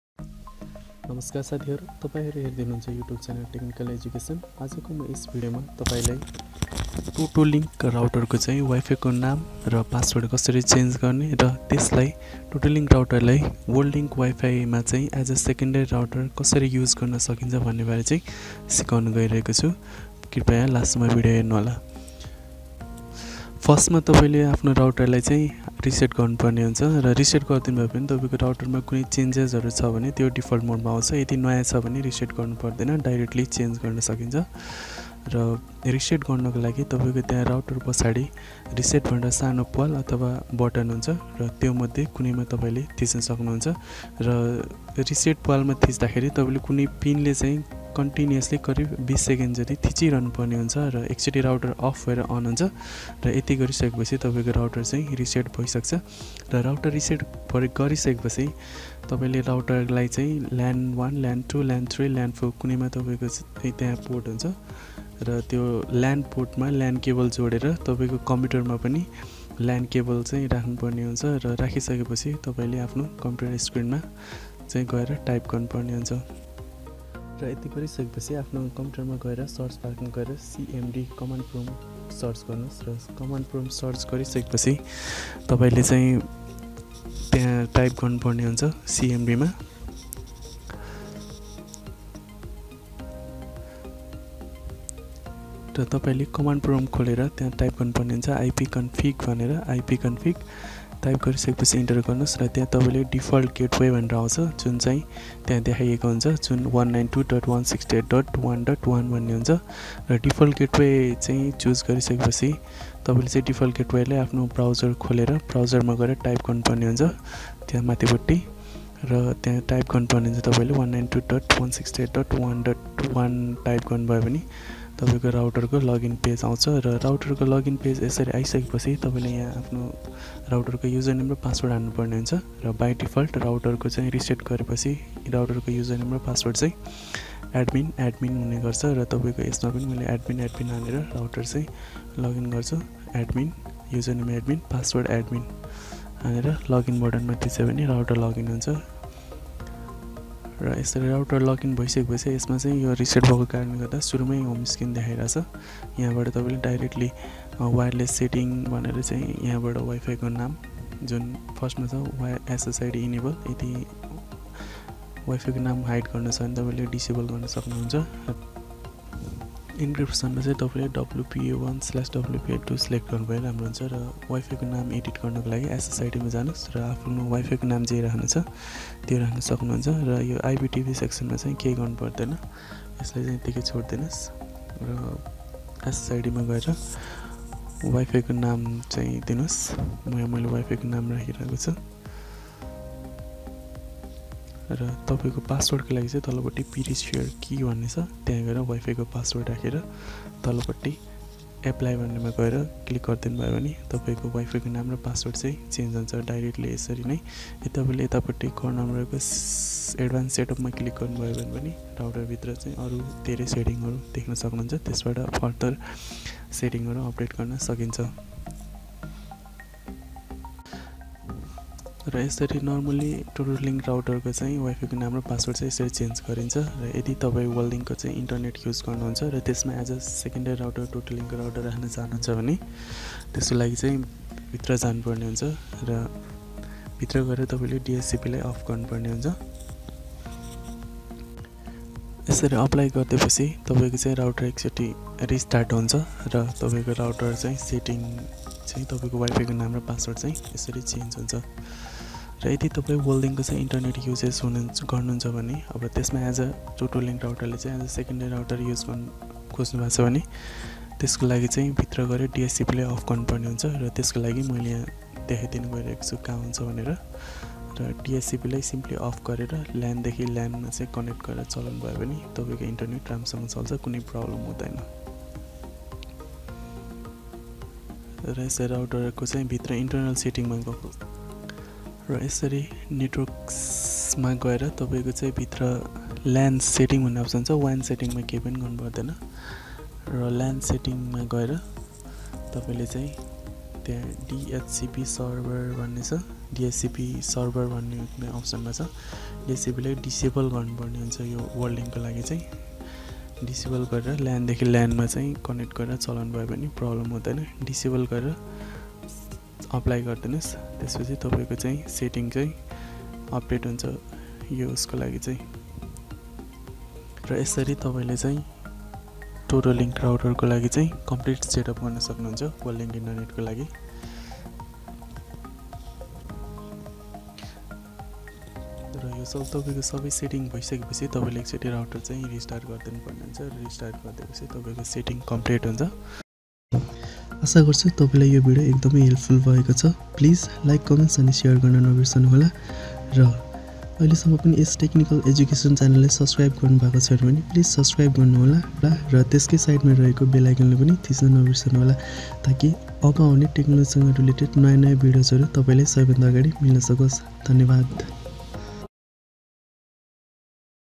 How to Configure TOTO-Link Router configuration for Worldlink Internet 2020?change Name & Password?
This video provide information about how we can change TOTO-Link Wi-Fi name and Password. How can we set up configuration for WordLink interne Connection in nepal.